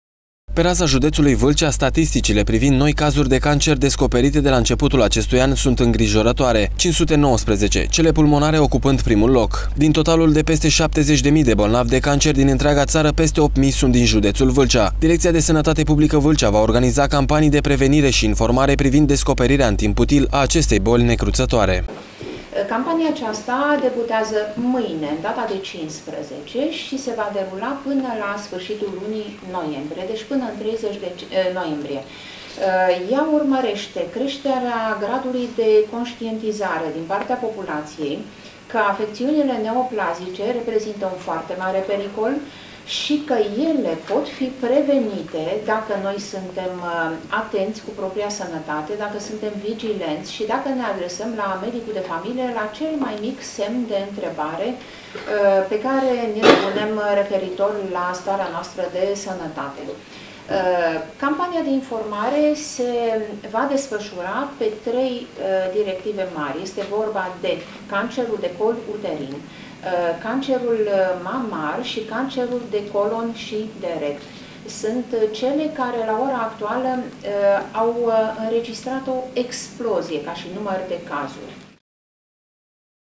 Campanie impotriva cancerului.mpg
Pe raza Judetului Valcea statistici privind noi cazuri de cancer descoperite de la inceputul acestui an sunt ingrijoratoare 519, cele pulmonare ocupand primul loc,  din totalul de peste 70.000 de bolnavi de cancer din intreaga tara peste 8000 sunt din Judetul Valcea. Directia de Sanatate Publica Valcea va organiza campanii de informare,  prevenire si descoperire in timp util a acestei boli necrutatoare. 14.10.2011